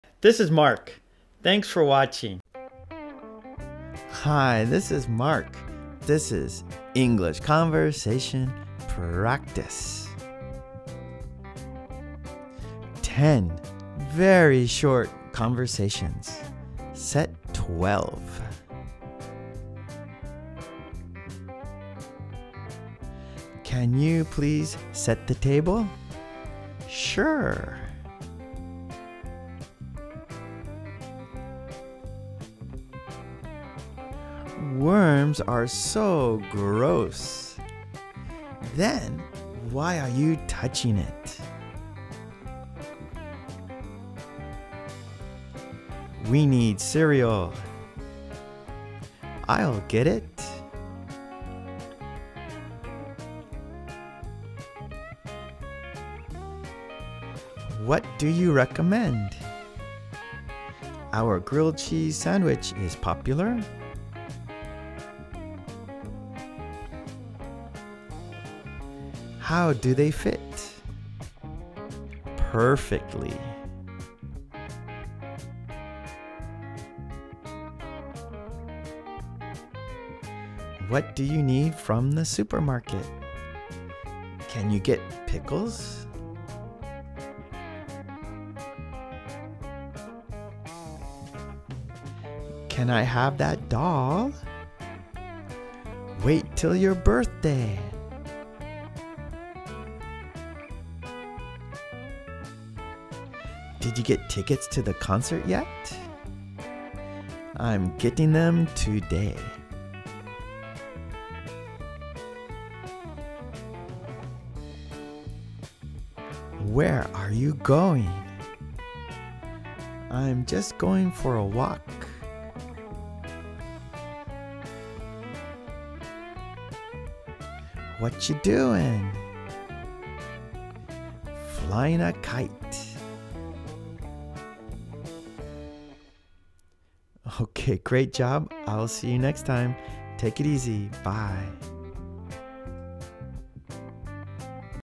10 Very Short Conversations | Set 12 | Easy English Conversation Practice | ESL
Here is my eBook on 25 Simple Short Conversations